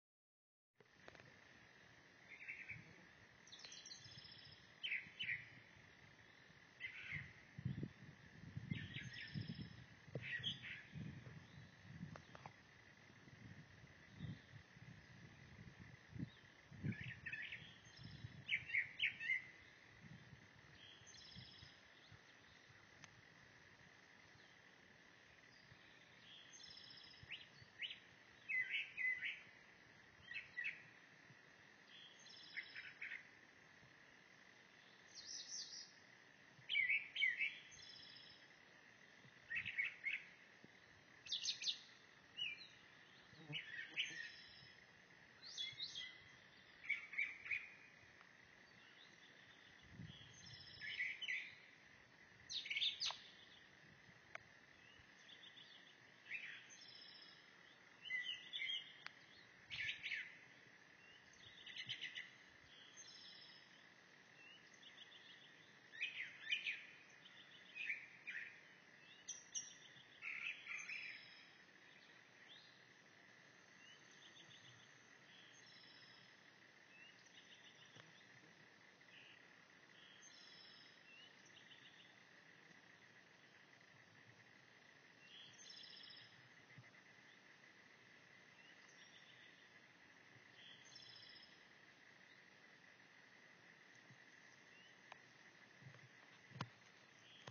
Tony McVay and Chrissy listen to a Mockingbird in early spring
Tony McVay and Chrissy are delighted that a Mockingbird has chosen the woods near their mountain cabin to nest in. I hope they stay. They don't usually have Mockingbirds at this elevation, it must be due to the early spring in Little Switzerland. Tony McVay and screenwriter/novelist Chrissy McVay look forward to seeing their chicks! The McVays feel truly blessed to live in the Blue Ridge Mountains surrounded by such beauty and hope to have more birding photos to share soon, though this guy is proving to be very camera shy :)